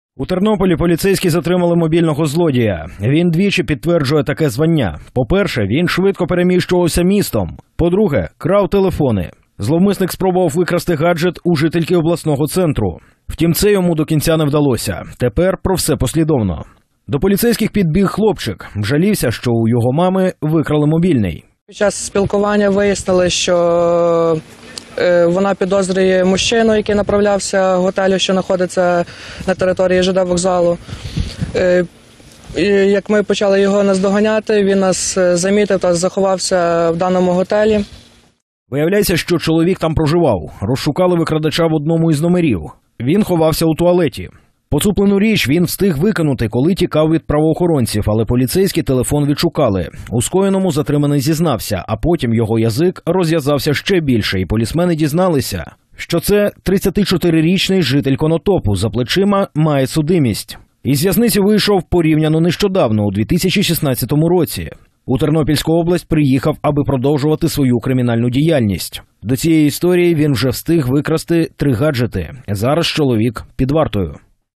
У Тернополі затримали крадія мобільних телефонів
Він двічі підтверджує таке звання, по-перше, він швидко переміщувався містом, по-друге – крав телефони. Зловмисник спробував викрасти телефон у жительки обласного центру. Втім, це йому до кінця не вдалося. Тепер про все послідовно. До поліцейських підбіг хлопчик, жалівся, що у його мами викрали мобільний…